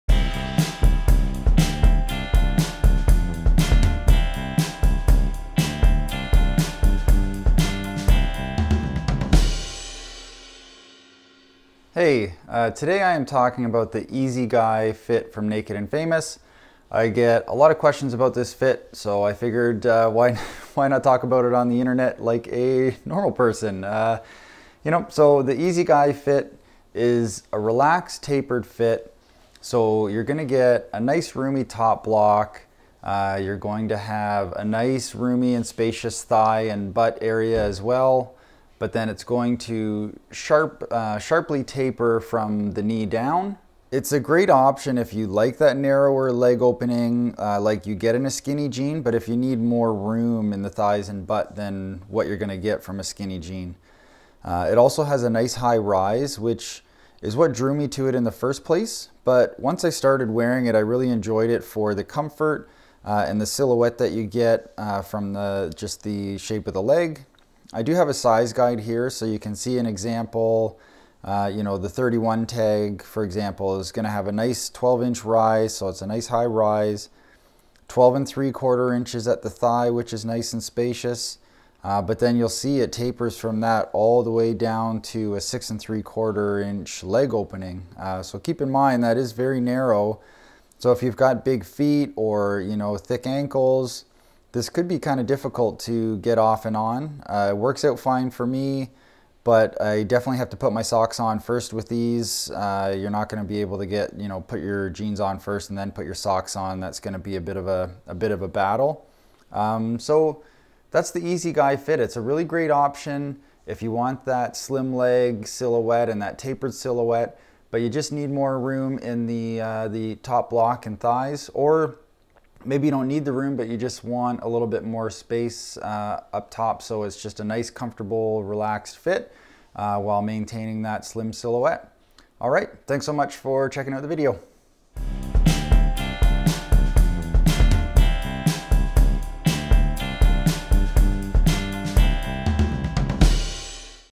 Naked & Famous: Easy Guy Fit
You've probably guessed I'm a big fan of Naked & Famous.  I have tried all their fits and the Easy Guy is my second favourite.  Nice and roomy but still gives a nice tapered silhouette.